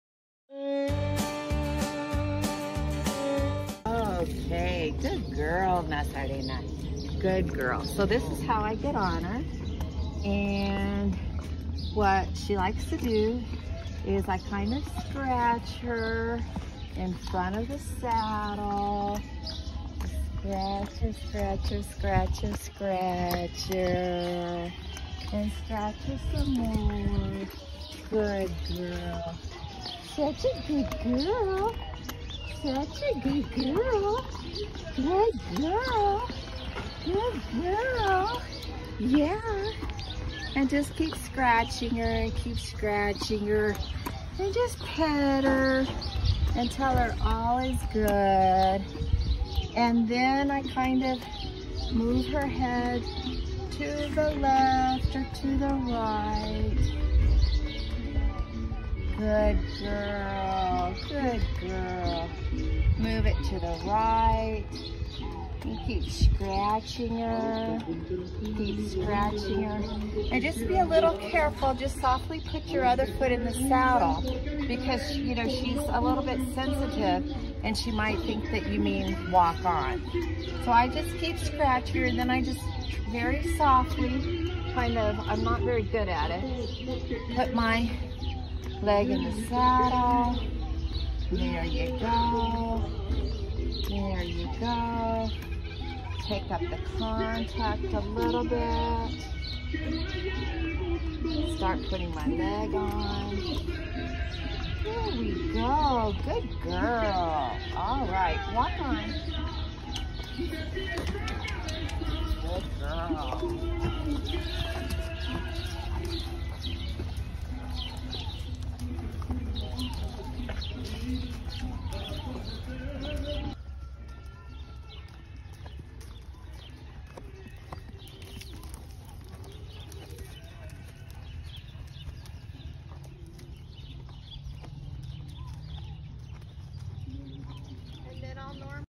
Stretching for riding / Estiramientos para montar
Te compartimos este tip para aplicar antes de iniciar a montar a tu caballo, esperamos que te funcione!

We share with you this tip to apply before starting to ride your horse, we hope it works for you!